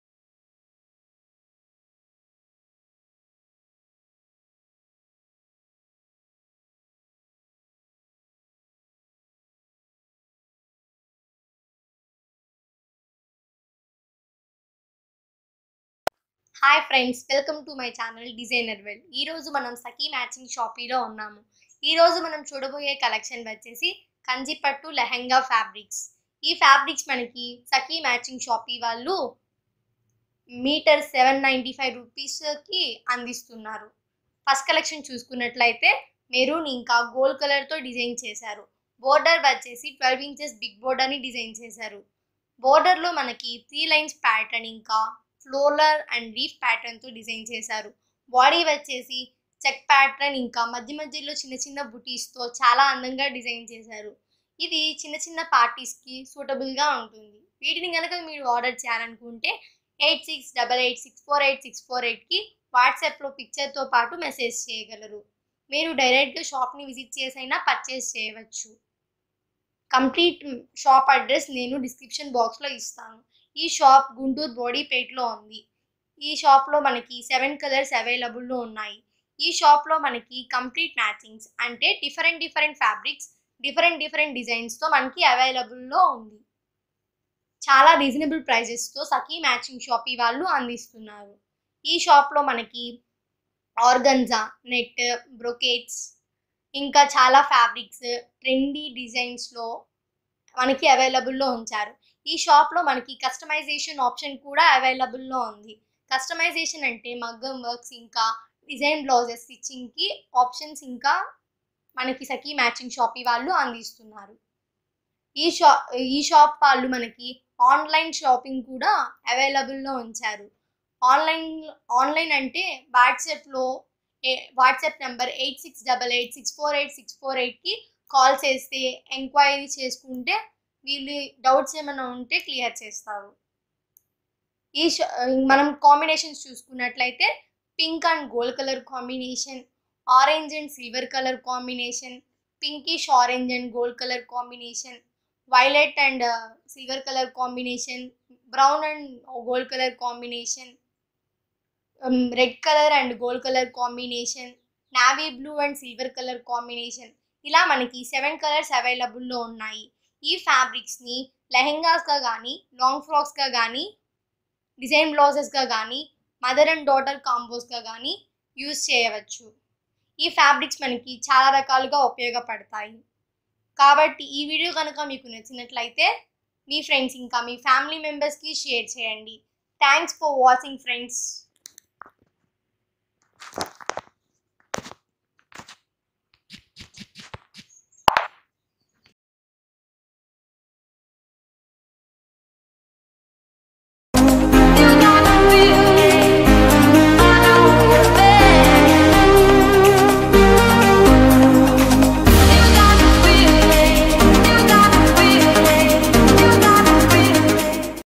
Latest Trendy Designs in Kanchi Pattu Lehanga Fabric to customize Long frocks With Price
SHOP ADRESS:

SAKHI MATCHING SHOPEE
5/25/36 RAMANJANA COMPLEX
3/7 BRODIPET, GUNTUR